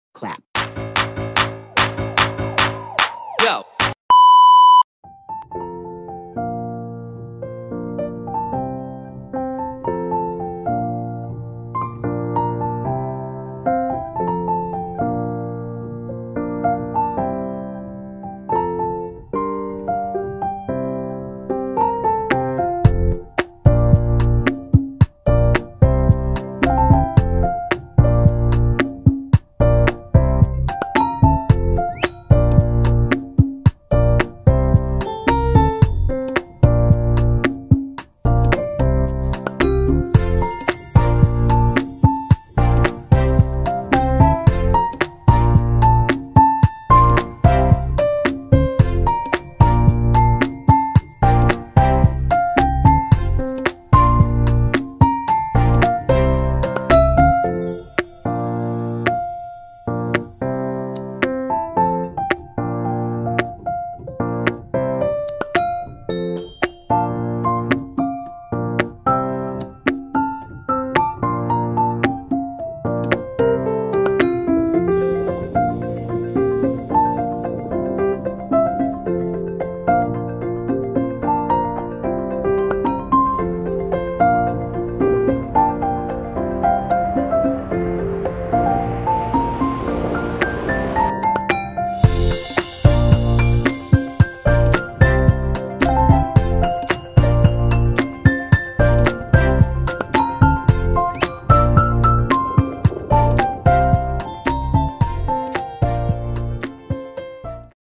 NORWEGIAN TYPICAL HOUSES I HOUSE STYLE IN NORWAY I HOUSE FASHION IN NORWAY I NORWEGIANS HOME
A short glimpse for Norwegian houses. A typical Norwegian houses are very simple yet solid and they almost has the same style, depends on house type.

This is just to give you an idea on how Norwegian houses looks like. Just remember that the cost of living in Norway is very expensive.